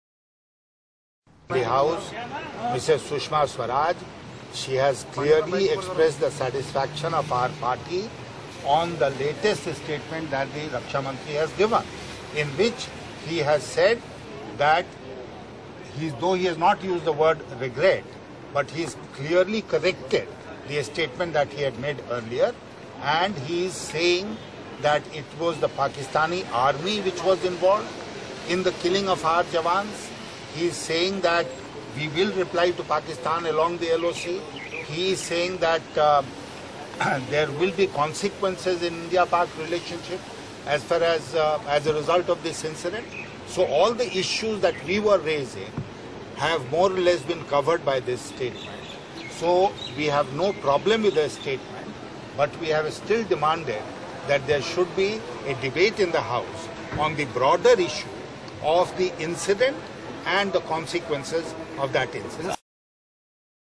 BJP is satisfied with the fresh statement of Defence Minister: Sinha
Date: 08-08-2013

BJP leader Yashwant Sinha has said that party is satisfied with the fresh statement passed by Defence Minister A.K.Antony in the issue of killing of fieve Indian solders.

He was commenting on the issue before media.